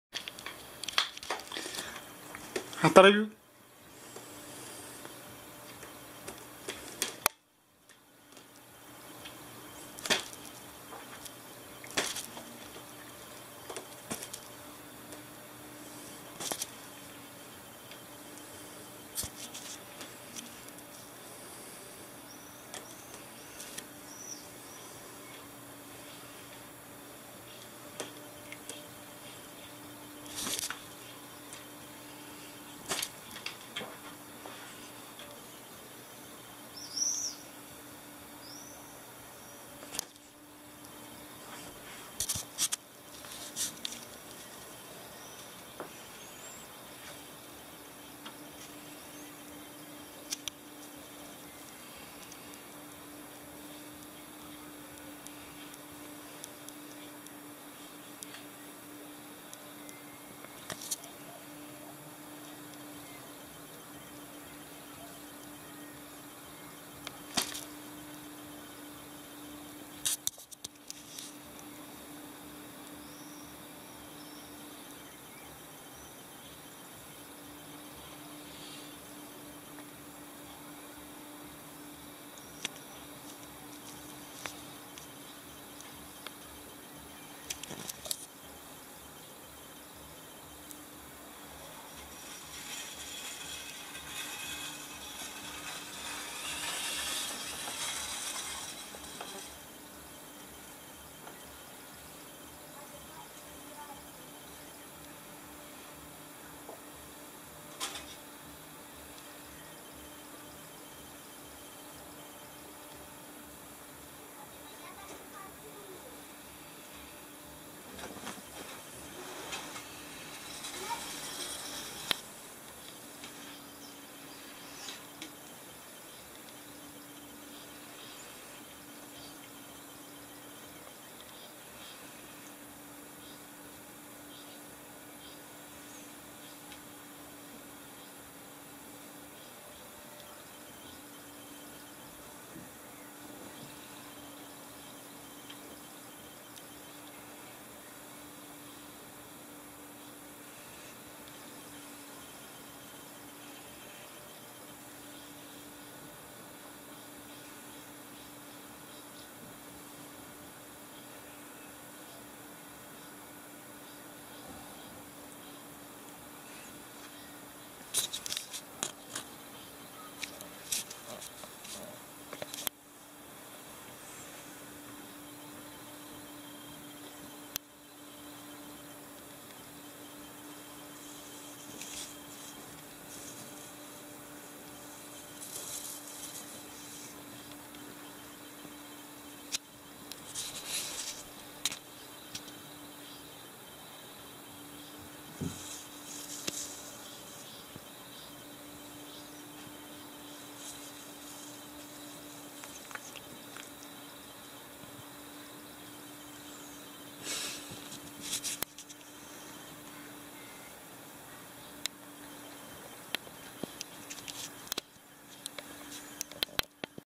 Охота на муху
Кот Кузя охотится за мухой Привет , зритель моего видео!